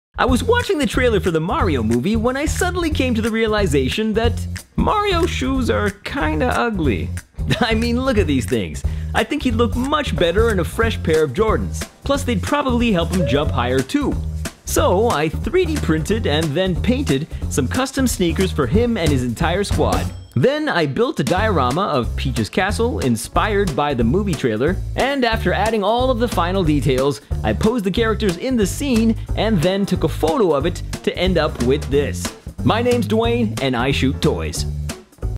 Mario Gets a Sneaker Upgrade!
Since the Super Mario Bros. movie and the Air Jordan movie both came out on the same day, I thought it would be fun to make a diorama that combines the two of them together!